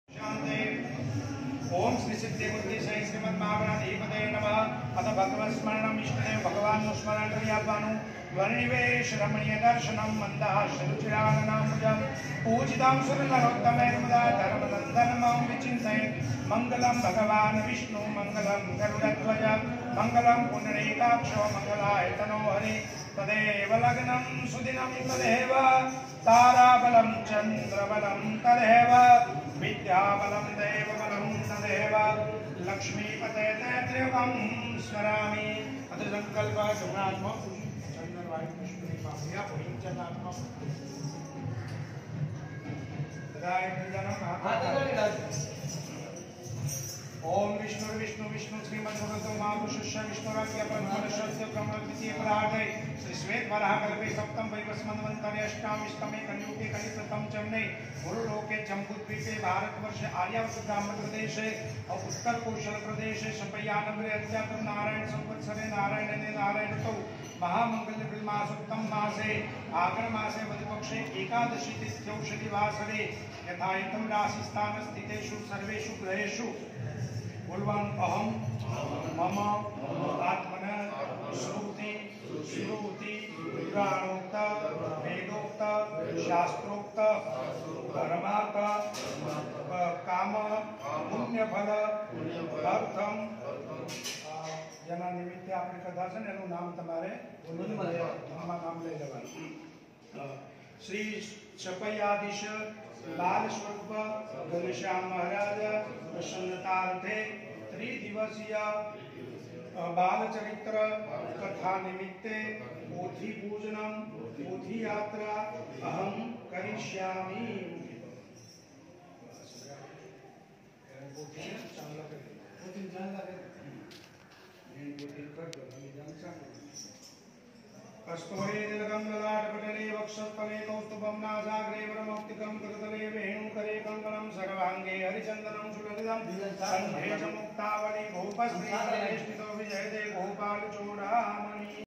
કથાની પોથીયાત્રા નું પૂજન કરતા સંતો તથા યજમાન પરિવાર 🙏🙌🙏🙌
ભુજ મંદિર ની પોથીયાત્રા ભુજના હરિભક્તો દ્વારા કથાનું આયોજન કરવામાં આવેલ છે કથાની પોથીયાત્રાનું પૂજન કરતા વડનગરના વિદ્વાન નવ યુવાન શાસ્ત્રી અભિષેક પ્રસાદ સ્વામી તે કથા ના વક્તા પણ છે 🙏🙌🙏